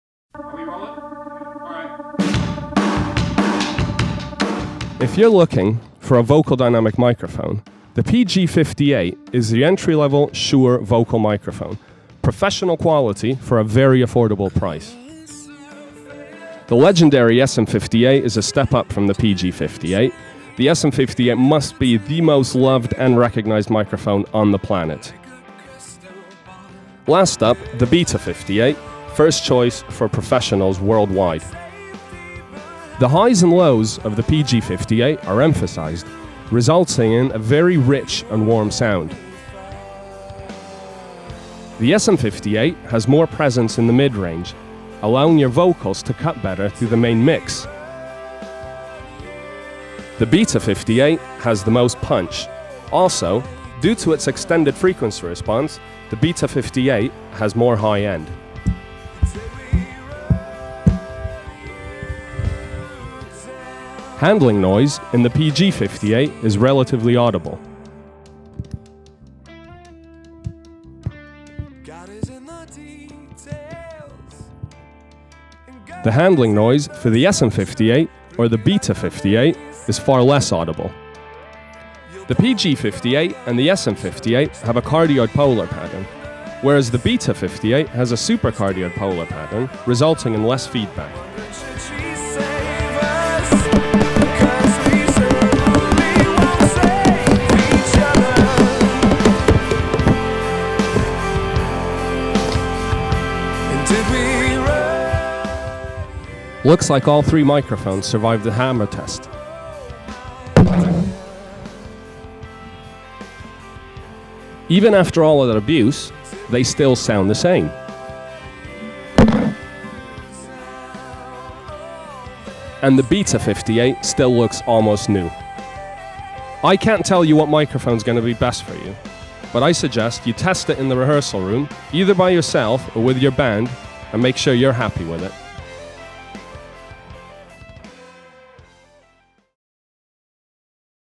比較: ボーカル用マイクロホン （PG58/SM58/BETA 58A）
ダイナミック型のボーカルマイクをお探しですか？PG58、SM58、BETA 58Aのマイクをサウンドを比較してご紹介します。